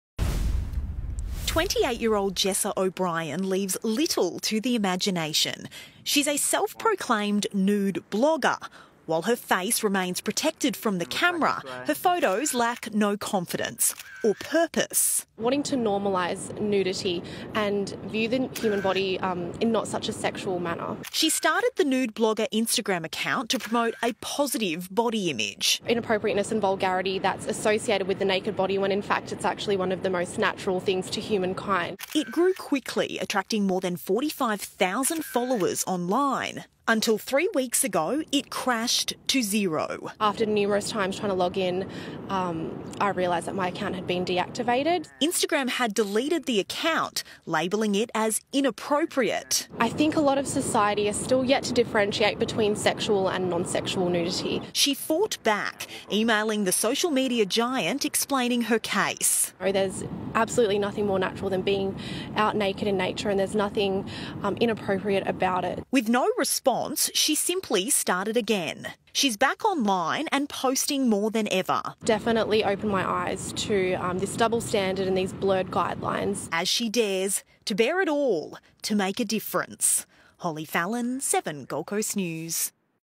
Mermaid woman promoting positive body image online
A young Mermaid woman who goes by the name of 'The Nude Blogger' is taking off her clothes online to promote positive body image.

See Less